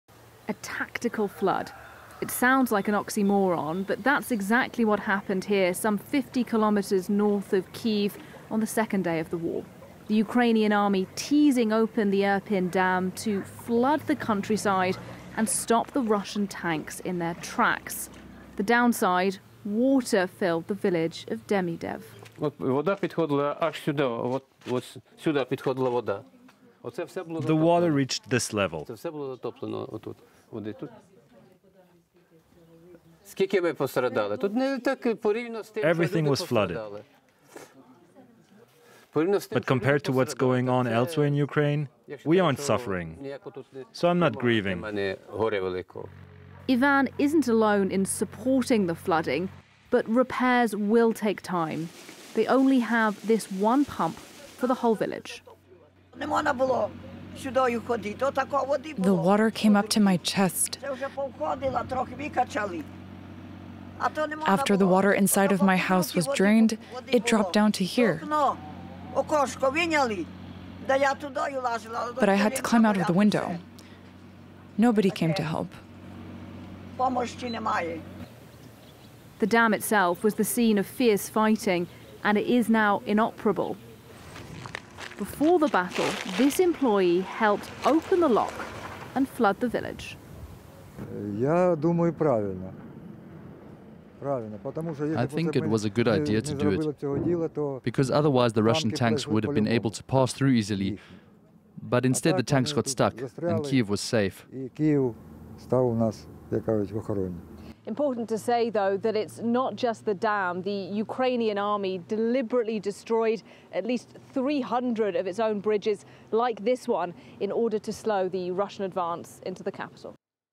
Ukrainian forces flooded village of Demydiv to hamper Russian advance on Kyiv • FRANCE 24 English
Military analysts are still debating why Russian forces failed to seize the Ukrainian capital at the start of the war more than two months ago. While the full response to that question is complex, one tactic that clearly did aid Ukrainian forces was self-sabotage. The Ukrainian army blew up key bridges and roads near Kyiv to hamper the Russian advance. In one case, Ukraine even flooded one of its own villages for the cause.